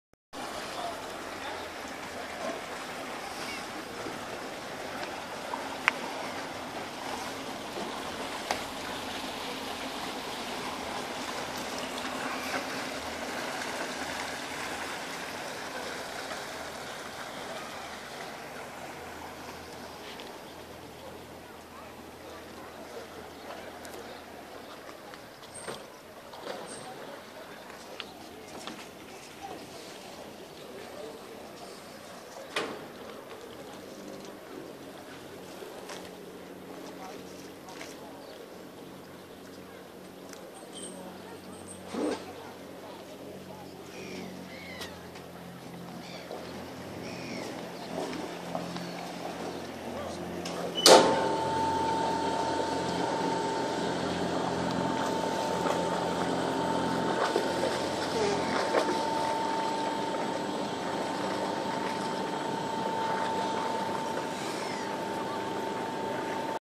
Andrei Rochian 3 years in England beautiful Classic Sail Yachts at Chichester Marina
Opening of the Gates and  beautiful  Classic Sail Yachts at  Chichester  Marina